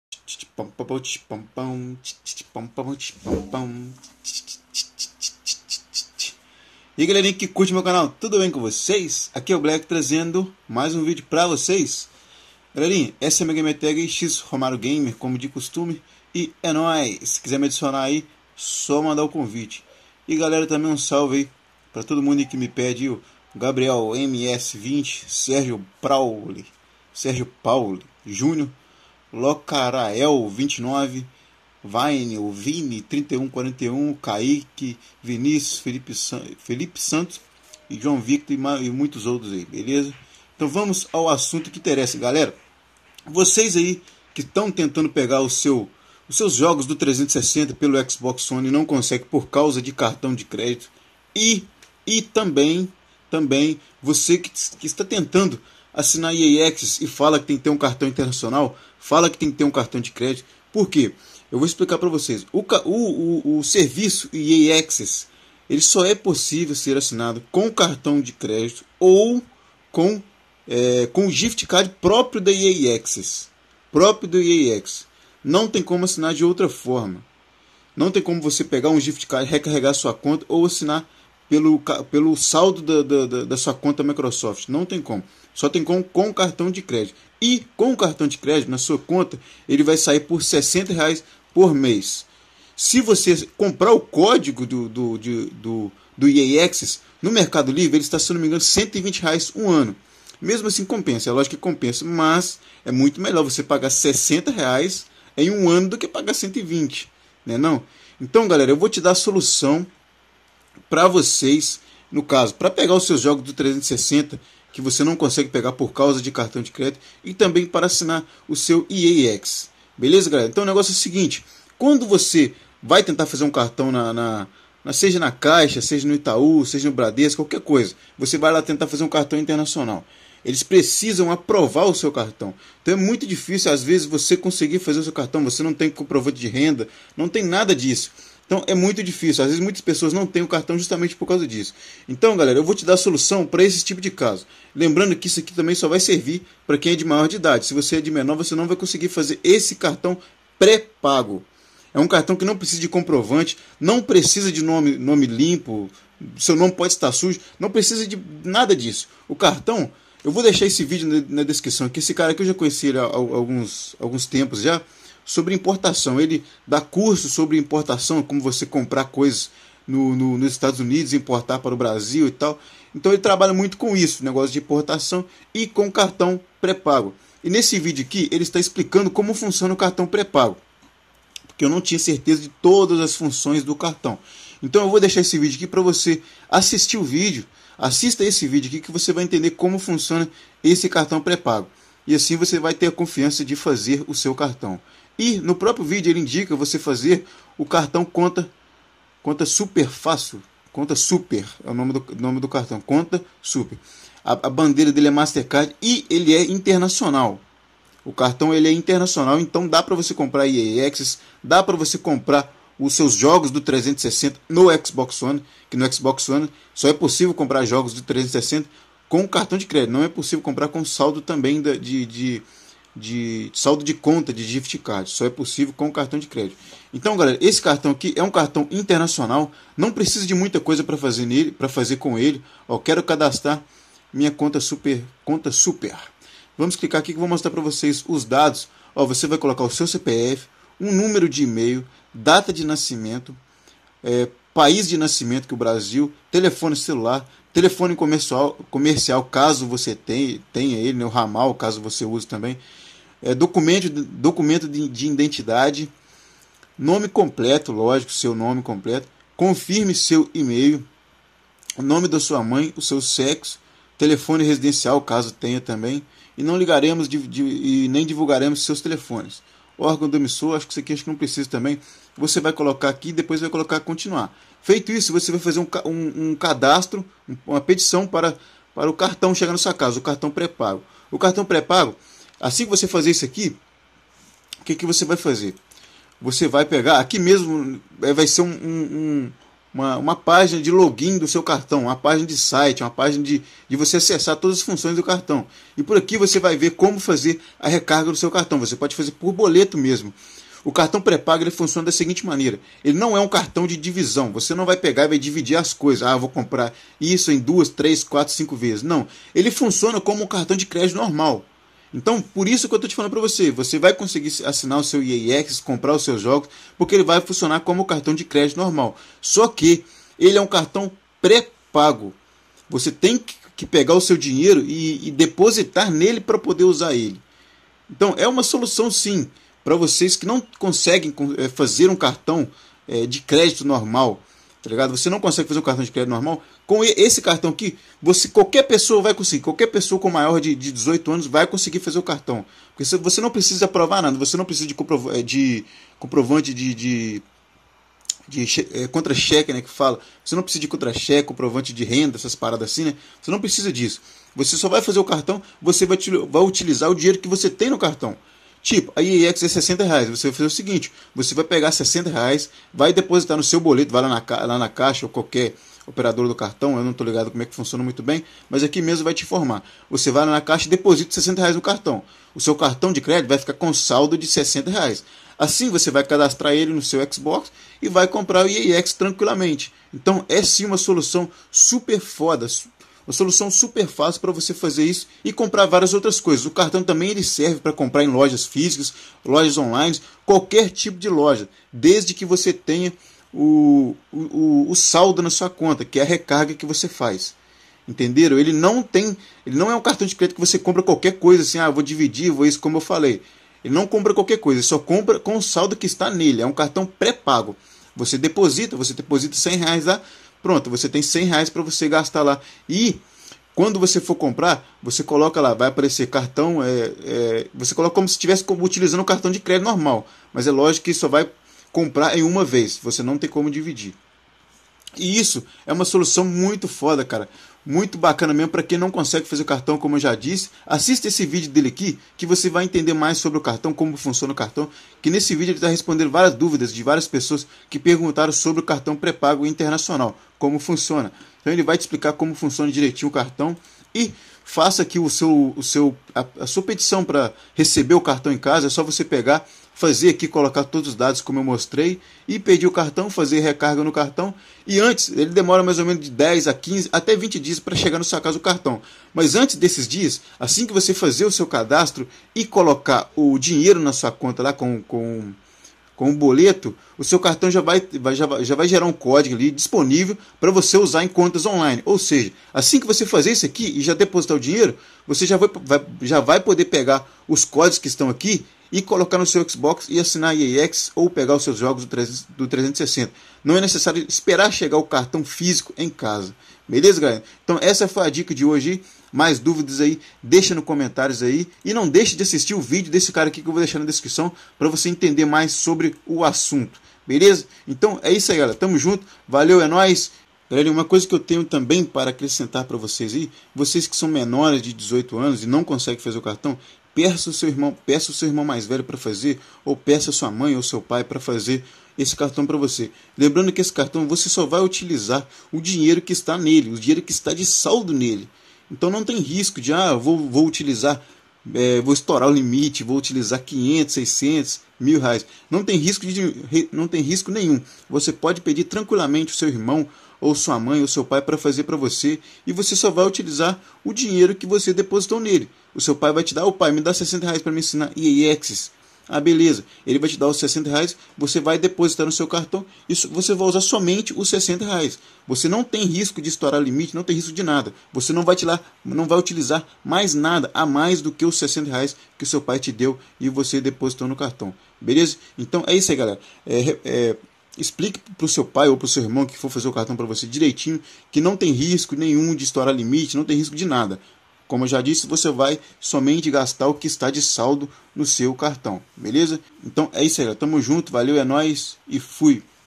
NÃO TEM CARTÃO DE CREDITO PARA EA ACESS? PEGUE TBM OS JOGOS DO 360 NO ONE
CONTATOS COMERCIAIS
romariogamer99@hotmail